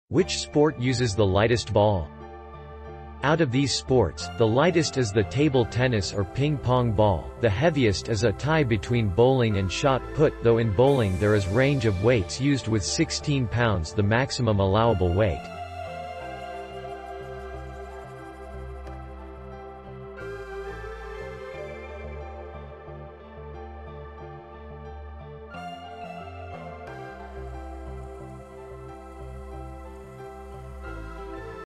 Which sport uses the lightest ball?
Out of these sports, the lightest is the table tennis or ping-pong ball, the heaviest is a tie between bowling and shot put, though in bowling there is range of weights used with 16 pounds the maximum allowable weight.